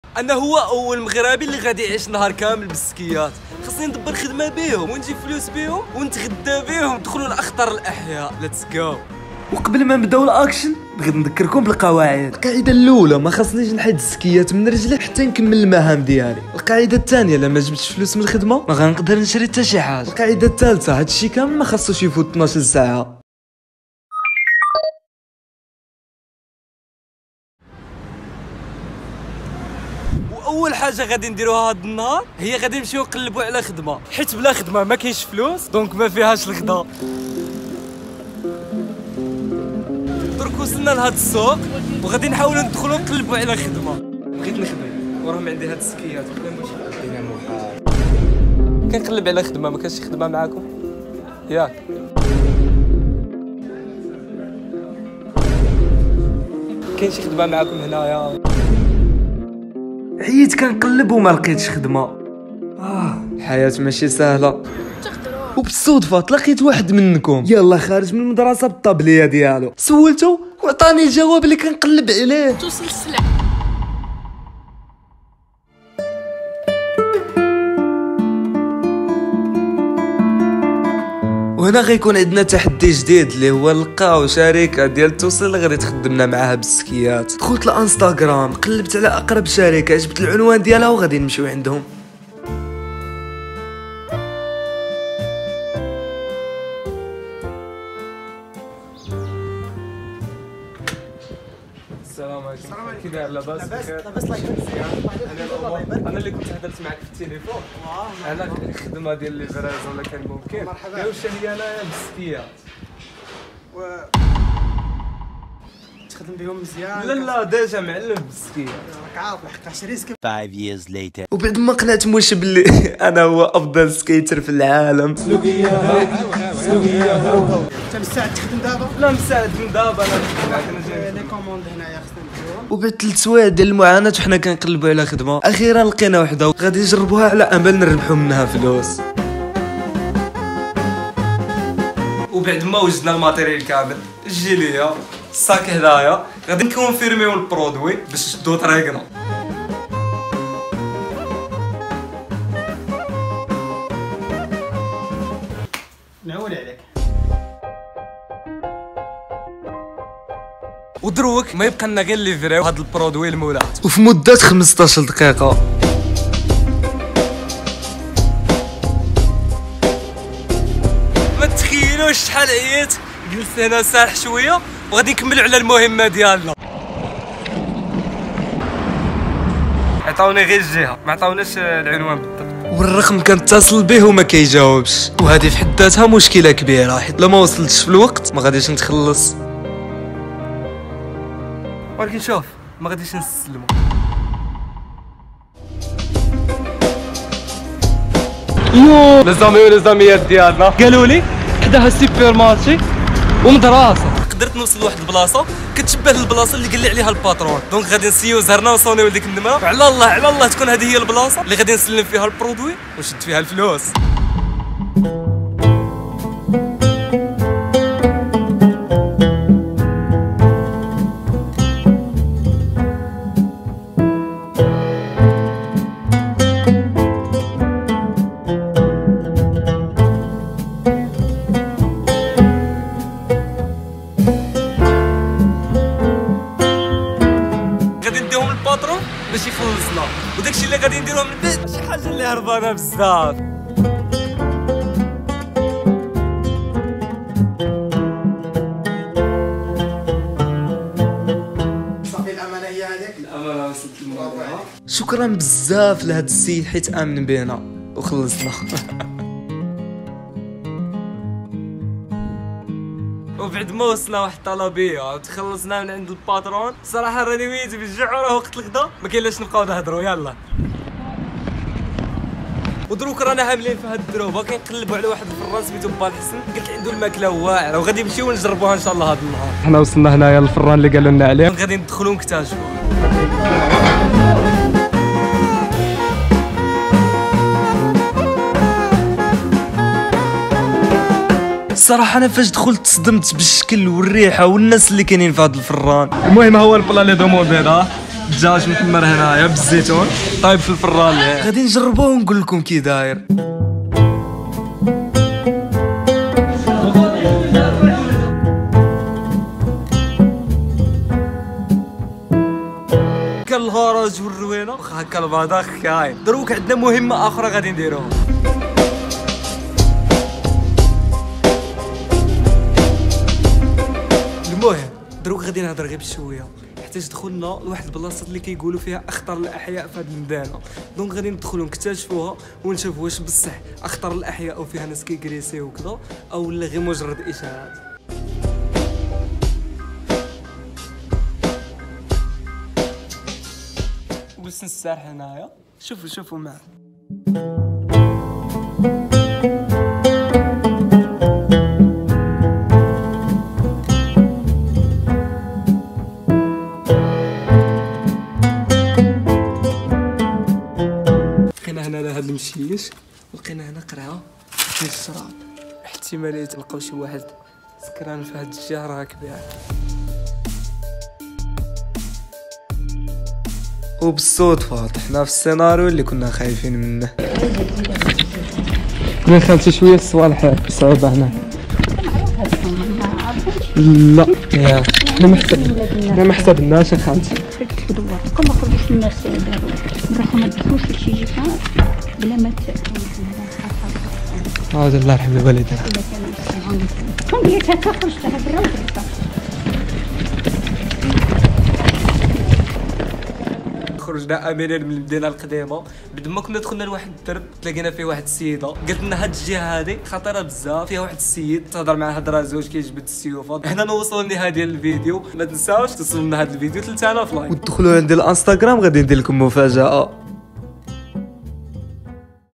عشت يوم كامل بالاسكيت 🚨🍔 BZAAAF
في هذا الفيديو ( تحدي )  عشت يوم كامل  بواسطة الاسكيت / سكيت ، جربت خدمة التوصيل  و عملت جولة أكل الشوارع 🍔🇲🇦 ...